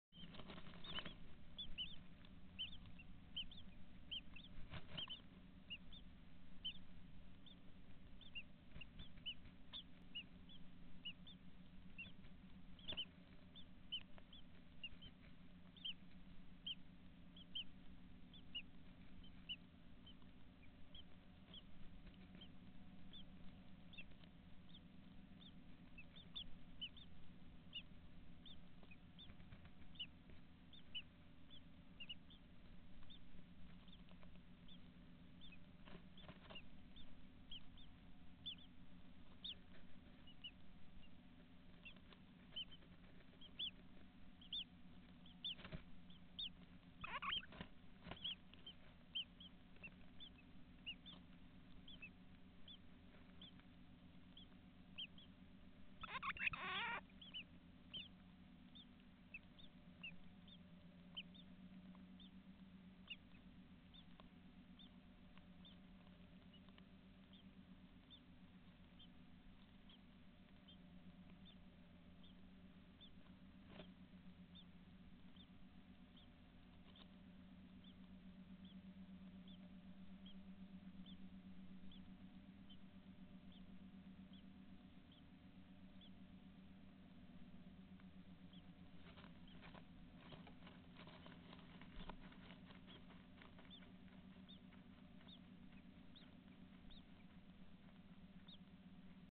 2019 05 11 13:22 feeds the youngest 0.52, starlings
Looduskalender webcamera pesakast. Estonia